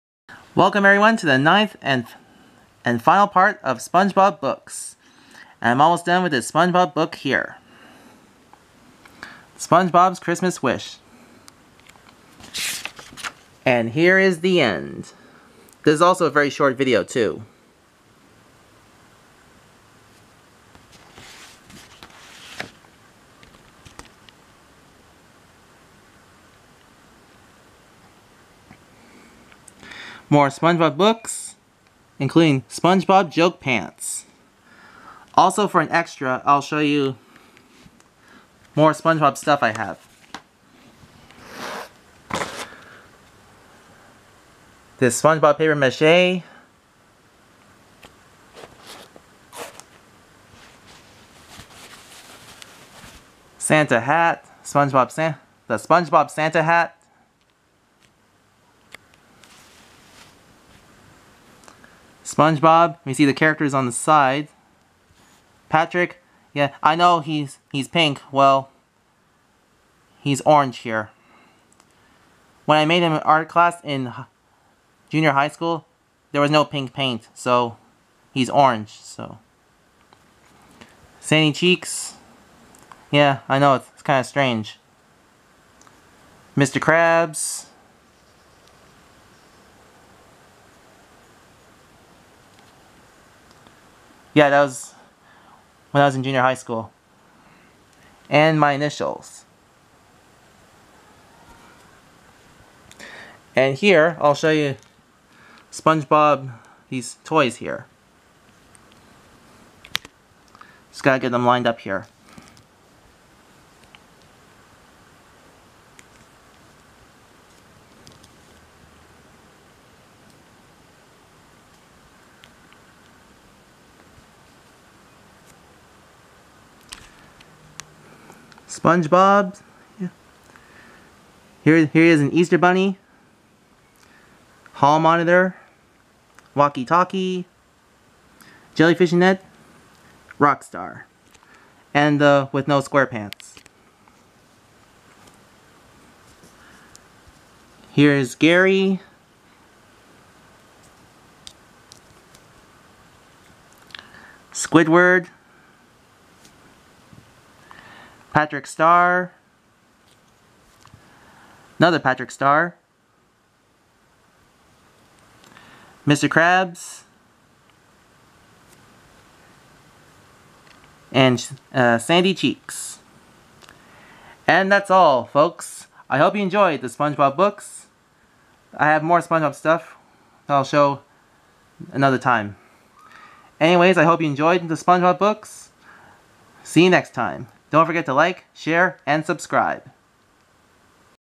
"SpongeBob Squarepants Books" (Part 9 of 9) (Final Part) ("Christmas Wish" and "Surprise Extra")
Here is the final part of "SpongeBob Squarepants books", in this part is the final part where I finish up "Christmas Wish"

0:01 "SpongeBob's Christmas Wish" (Rest of the book)
0:43 "SpongeBob Stuff" (from start to end of this video)

At the end you'll hear me say "I have more "SpongeBob stuff" that I'll show another time, well if you watch the previous "YouTube videos" that I did like "WDF To The Rescue", "WDF To The Rescue 2" and other videos that I did in the past you'll since those videos with "SpongeBob" and characters in it, I hope you enjoy the videos, don't forget to Like, Share and Subscribe.